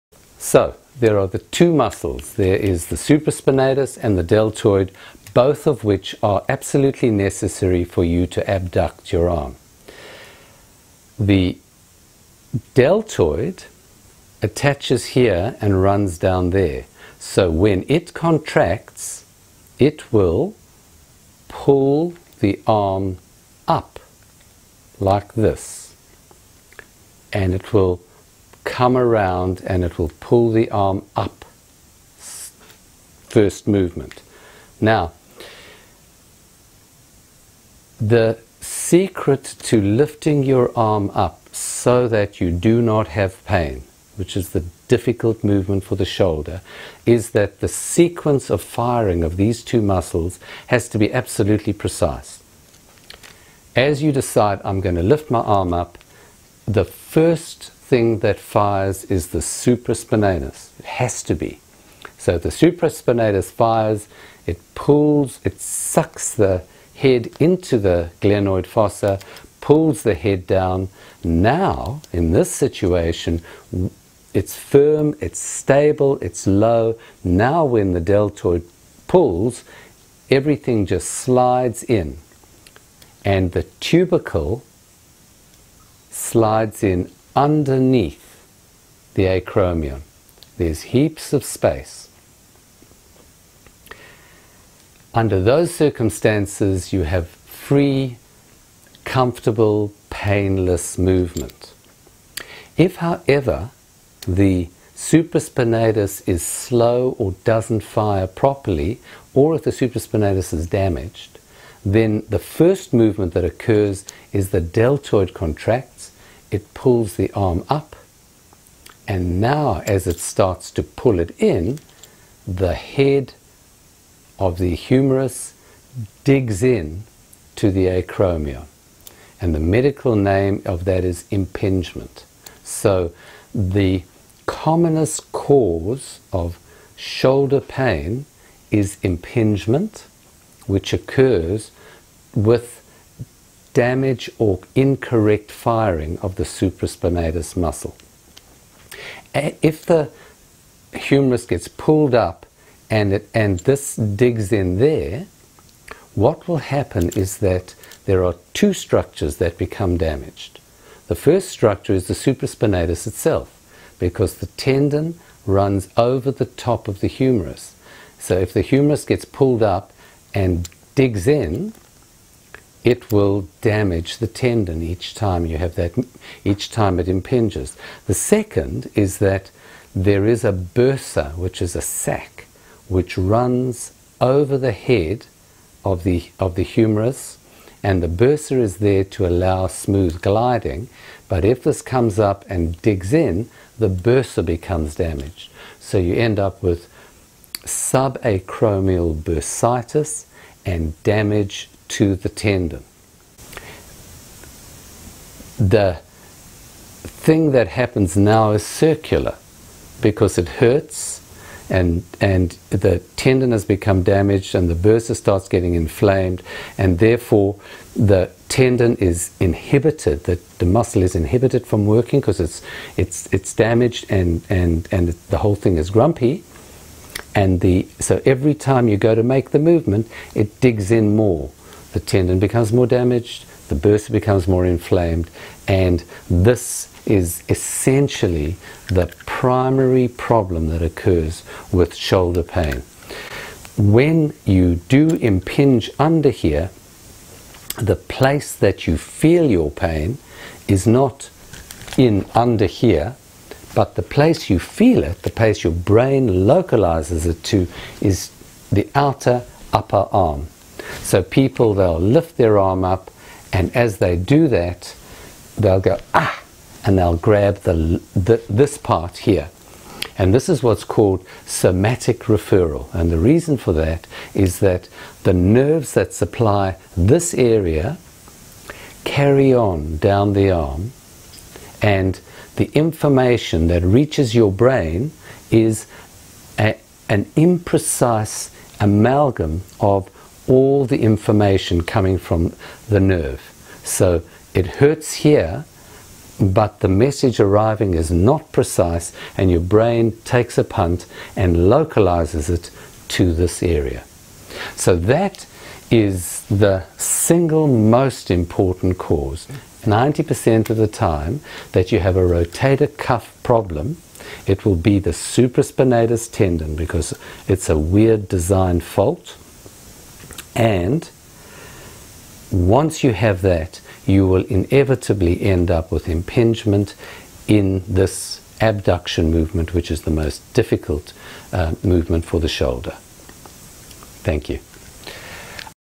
Shoulder Impingement and Common Chronic Shoulder Pain Explained
Subacromial impingement syndrome is the most common shoulder complaint in orthopedic clinics (44–65%) and the most frequent cause of shoulder pain in over-arm athletes.